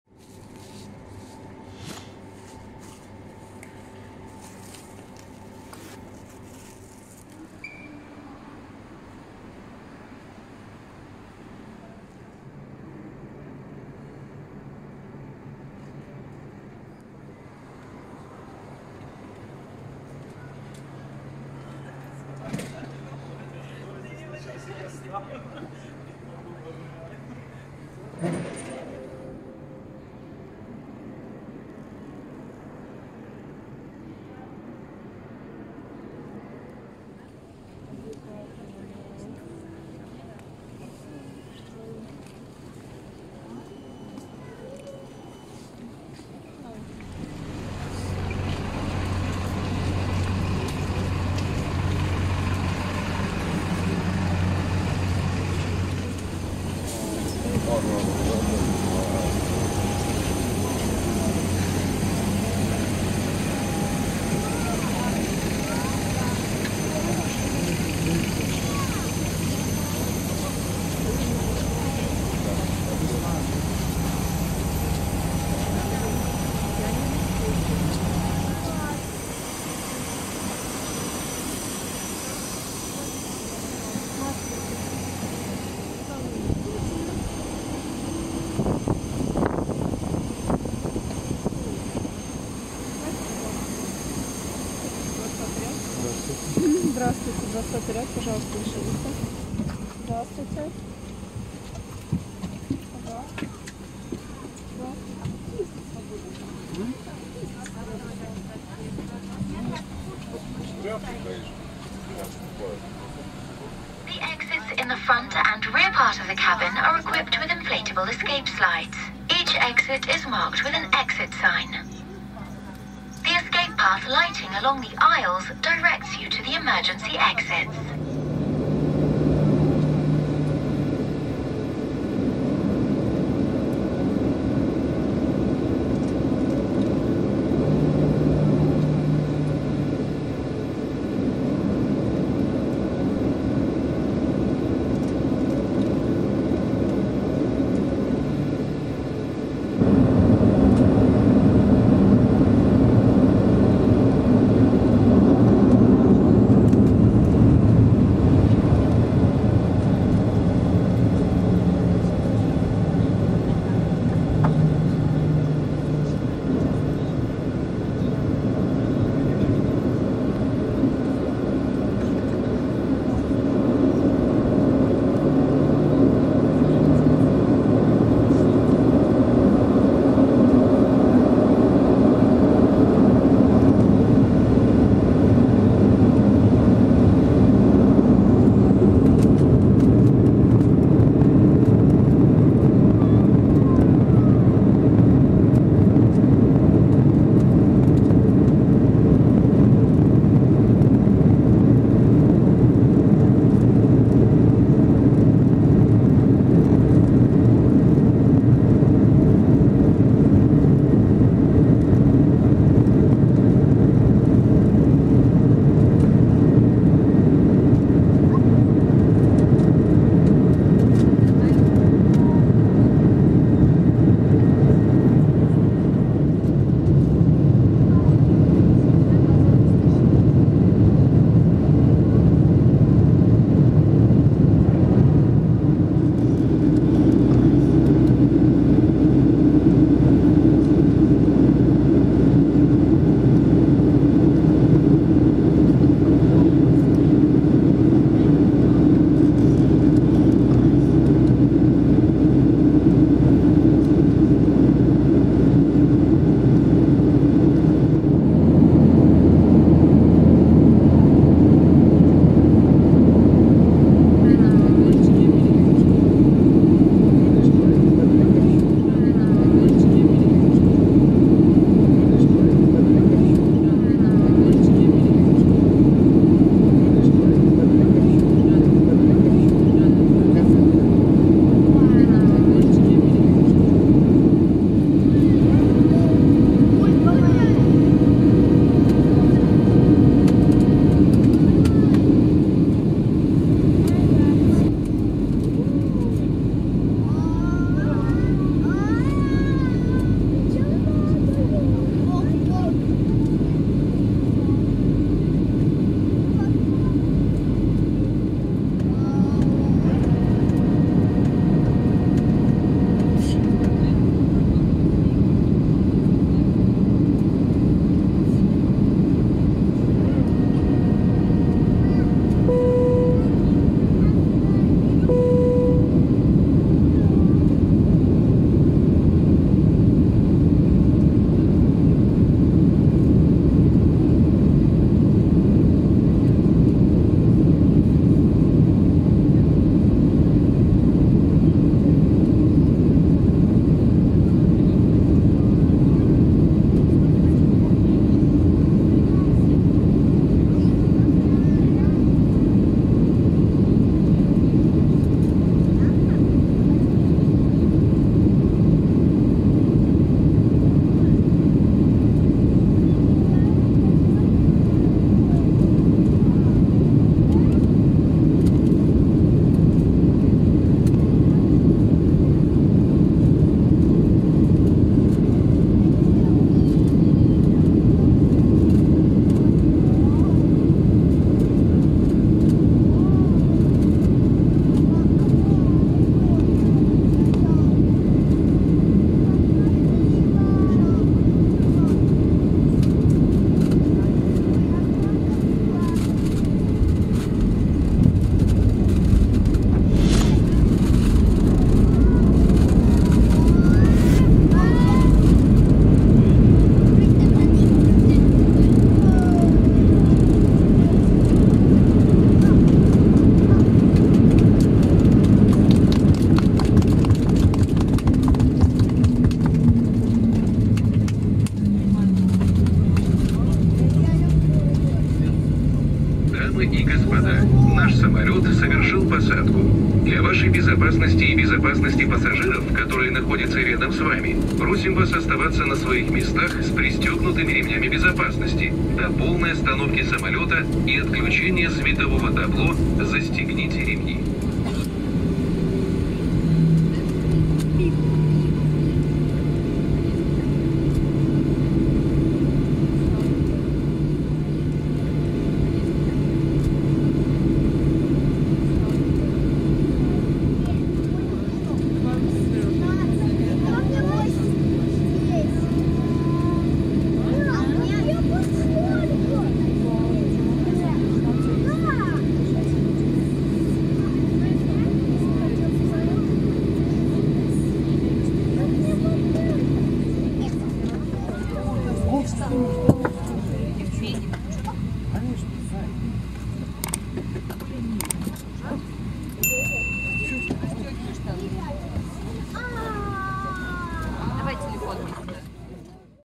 Boeing 737-8Q8 / VQ-BAL / Azur Air. Полет по маршруту Москва - Сочи.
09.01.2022. Полет по маршруту Москва - Сочи. Рейс ZF301 авиакомпании Azur Air.